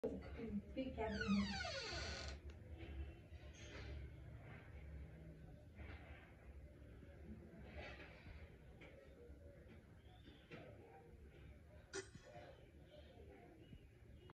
Food burst for fishes in aquarium
The fishes were feasting on the grinder shrimp food ..